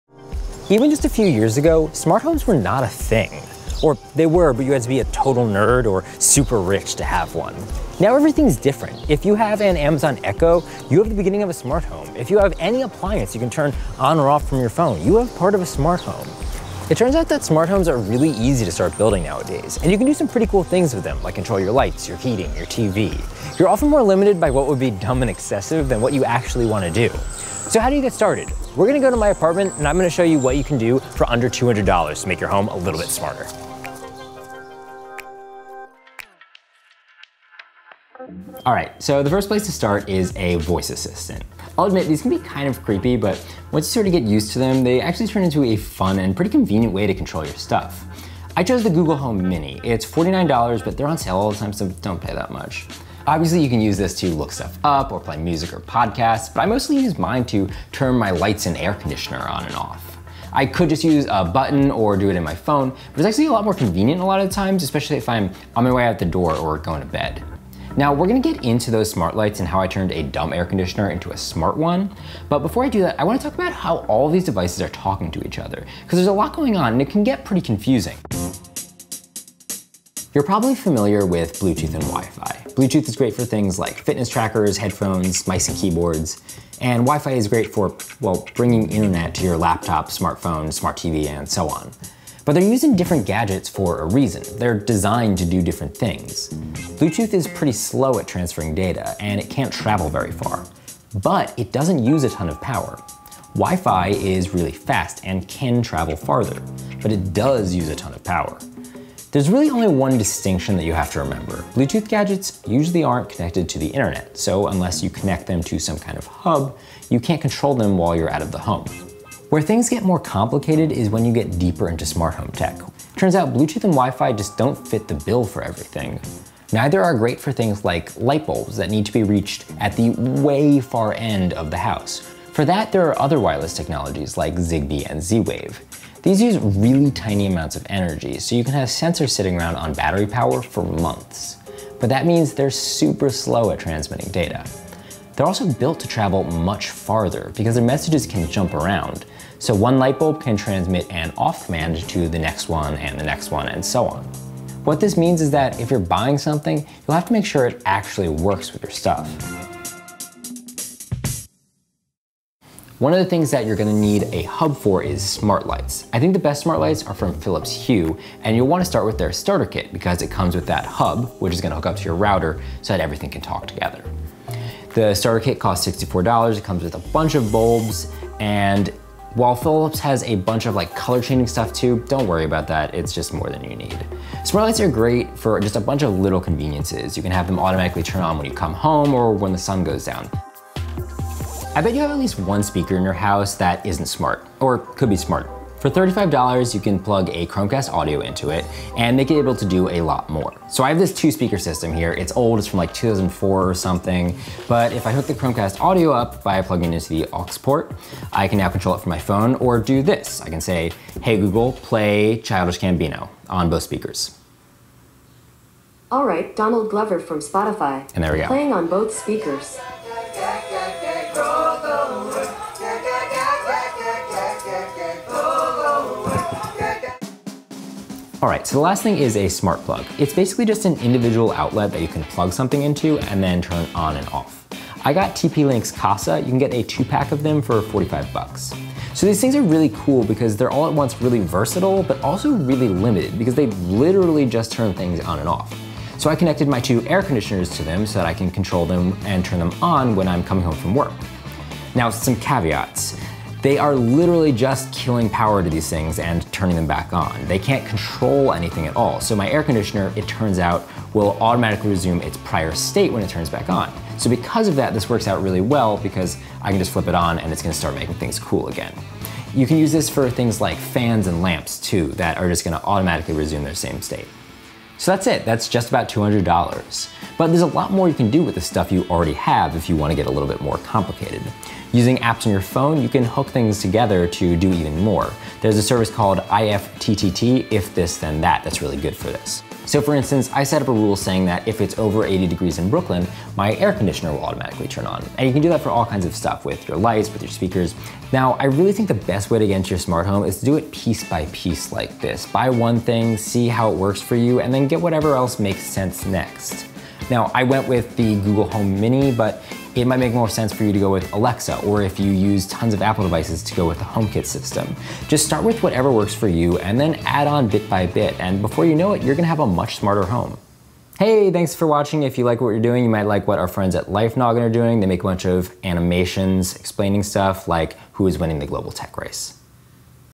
How to make a smart home for $200
How can you start making your home a little smarter? Here are some first steps you can take to start building smart tech into your home for under $200. We went with the Google Home, Philips Hue, Chromecast Audio, and TP-Link Kasa, but there's a whole lot more you can do.

Philips Hue Starter Kit
Kasa Smart WiFi Plug